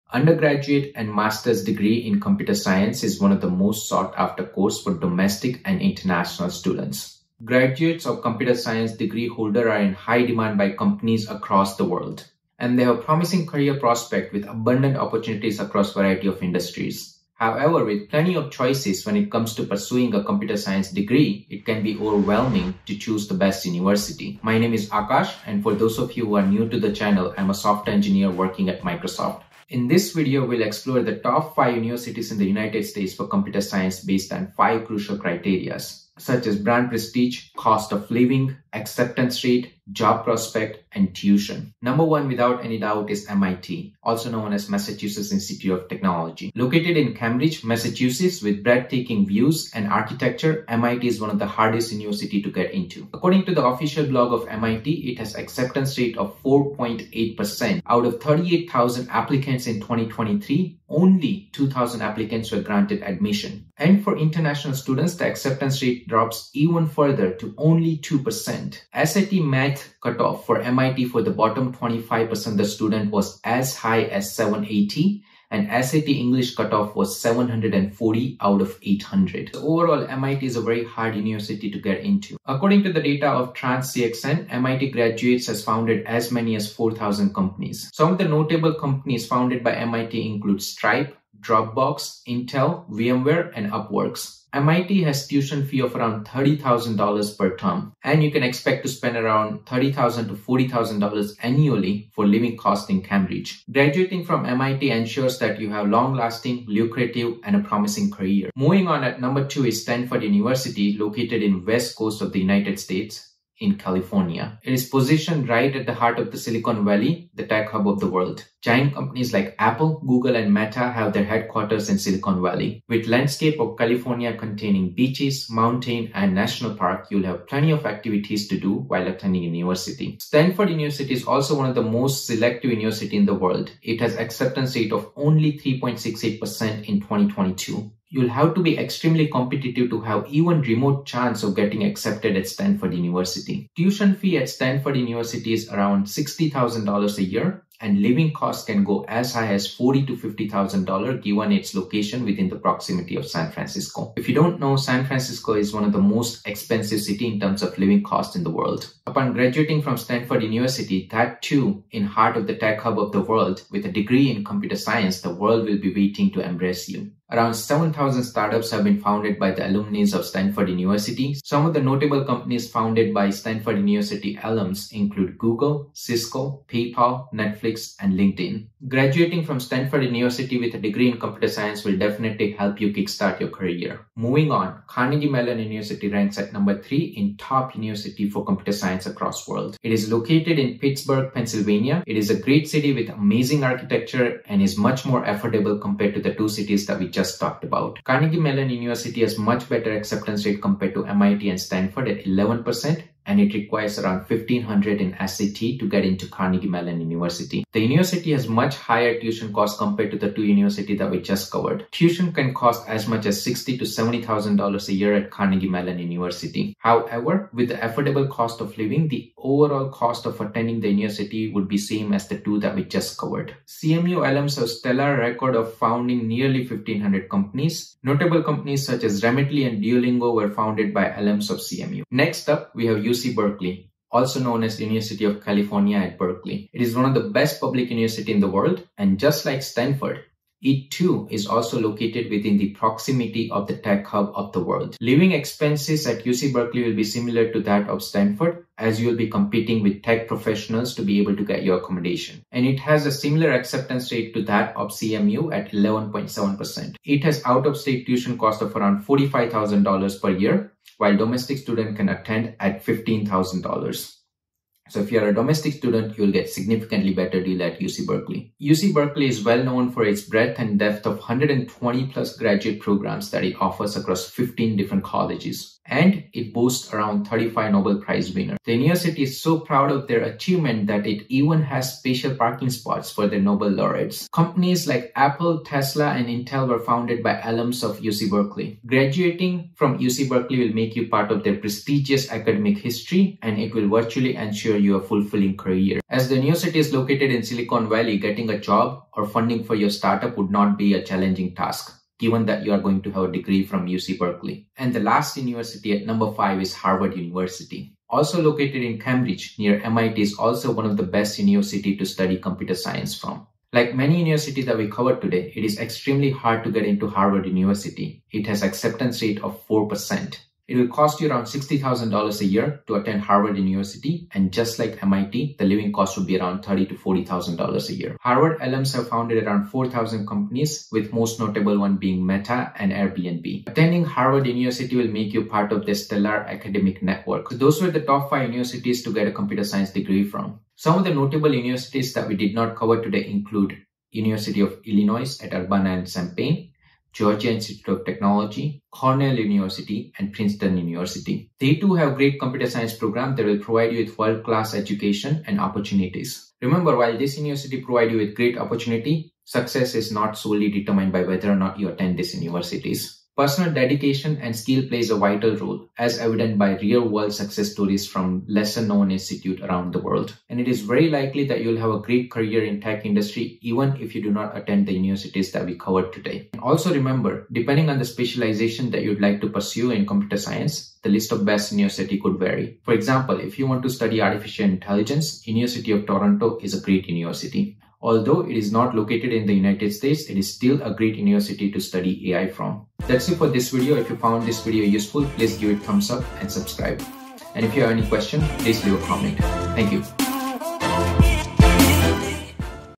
Top 5 University for Computer Science [2024]
Perusing a computer science degree is a shared aspiration for many students, driven by the promising career prospects as software engineers. In this video we will cover the top 5 best universities for studying computer science.

MIT (Massachusetts Institute of Technology)
Stanford University
Carnegie Mellon University
UC Berkeley (University of California at Berkeley)
Harvard University

However, success isn't solely determined by the university you attend; skills and dedication play crucial roles. A degree from these prestigious institutions opens doors to a plethora of career opportunities, thanks to their brand prestige and extensive academic networks.

Beyond these top five, there are other excellent options like the University of Illinois at Urbana and Champaign, Georgia Institute of Technology, Cornell University, and Princeton University. The choice may vary based on specializations within Computer Science – for AI/ML, LLMs, and Data Science, the University of Toronto stands out.

Chapters:
0:00 Introduction
0:41 MIT (Massachusetts Institute of Technology)
1:48 Stanford University
3:04 Carnegie Mellon University
3:52 UC Berkeley (University of California at Berkeley)
5:05 Harvard University
5:40 Notable Universities to Study Computer Science From
5:57 Conclusion

Note: The information shared in this video is based on my personal research and perspectives. Video represents my views only.